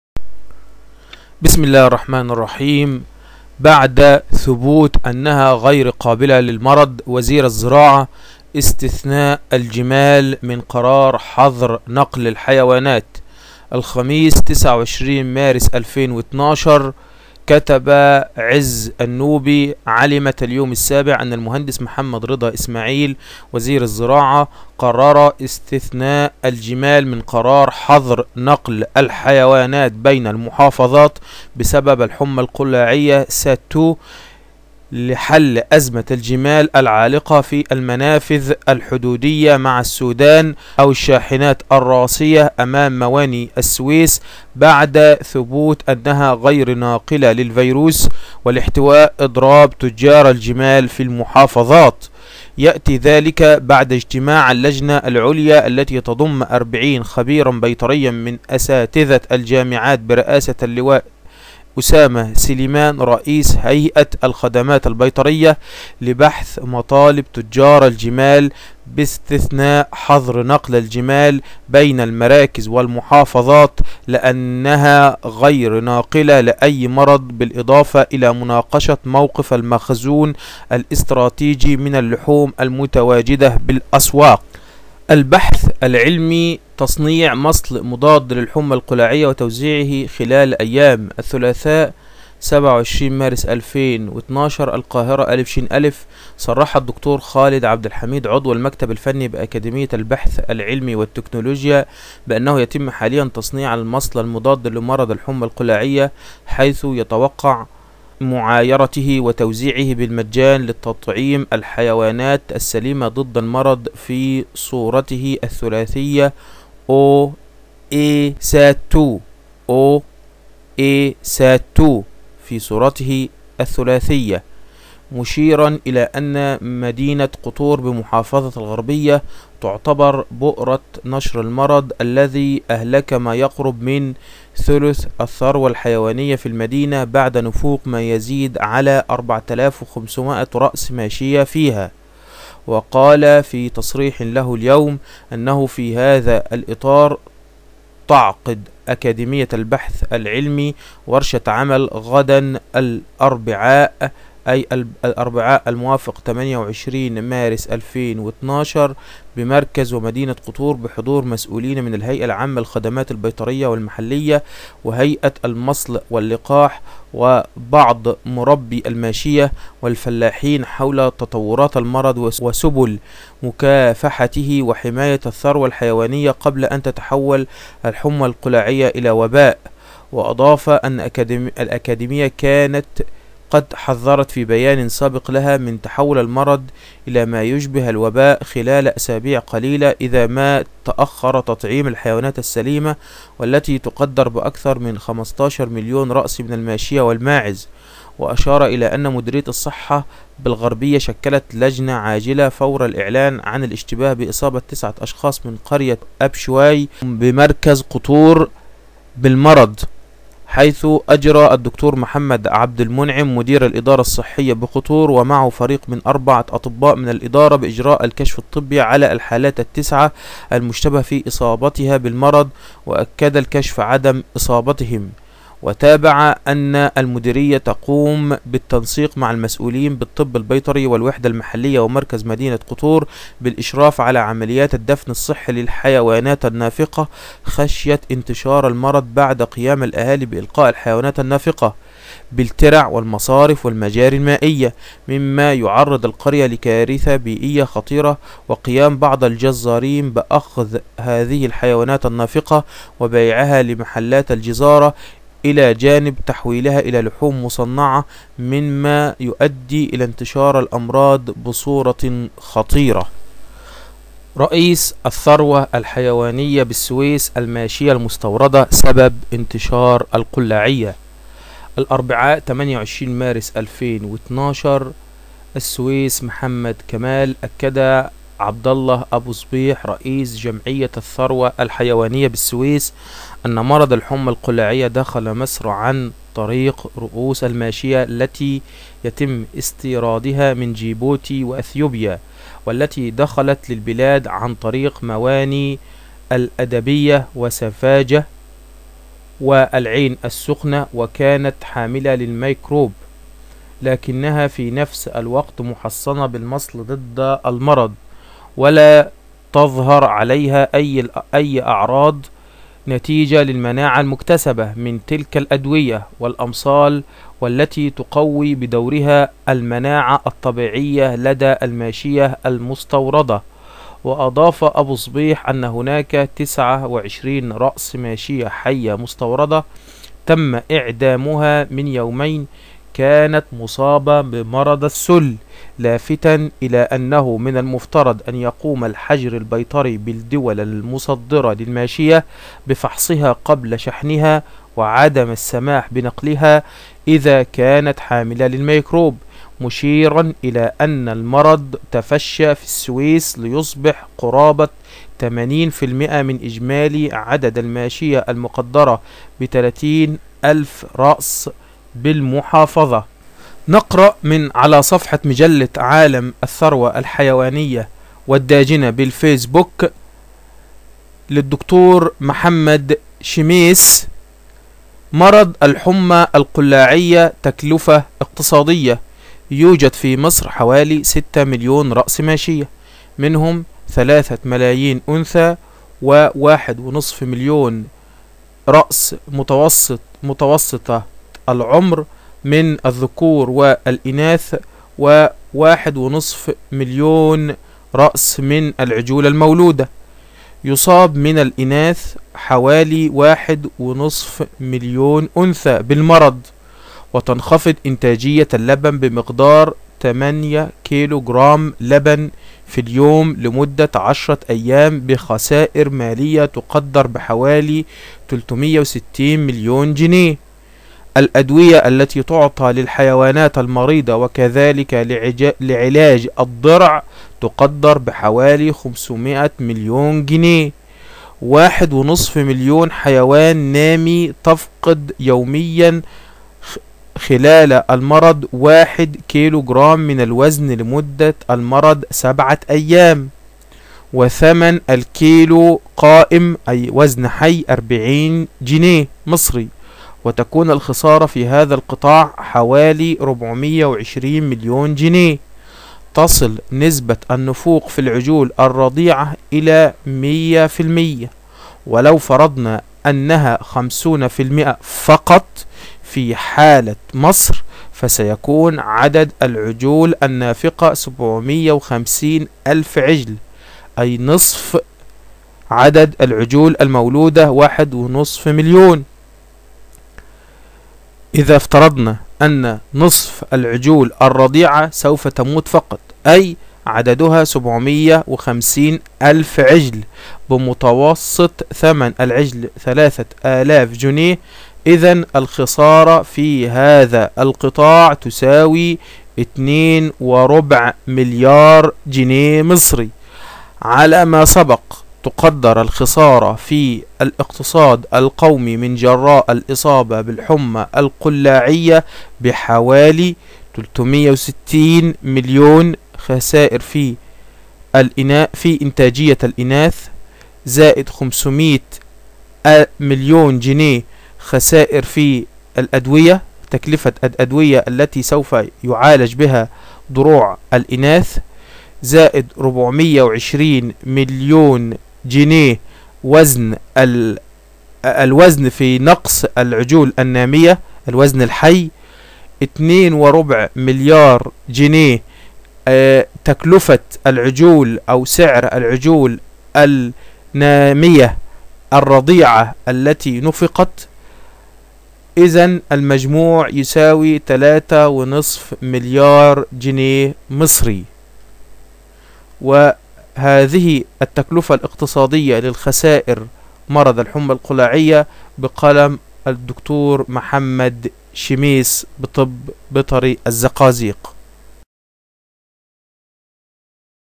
الحمى القلاعية 4 | دورة تدريبية في التسمين | حلقة 52
هذا هو الجزء (52) من (الدورة التدريبية في تسمين حيوانات المزرعة) نتطرق في تلك الحلقة إلى موضوع (الحمى القلاعية..ج4)، المصادر:
• وزير الزراعة: استثناء الجمال من قرار حظر نقل الحيوانات، اليوم السابع، الخميس 29 مارس 2012.
• البحث العلمي..تصنيع مصل مضاد للحمى القلاعية وتوزيعه خلال أيام: شبكة الإعلام العربية "محيط"، 27 مارس 2012.
• رئيس الثروة الحيوانية بالسويس: الماشية المستوردة سبب انتشار "القلاعية"، اليوم السابع، الأربعاء 28 مارس 2012.
• الحمى القلاعية..تكلفة اقتصادية/ بقلم د.محمد شميس -- صفحة مجلة عالم الثروة الحيوانية والداجنة على الفيسبوك.
قراءة وإخراج/ محمود سلامة الهايشة.
Training course in fattening Farm animals..Part 52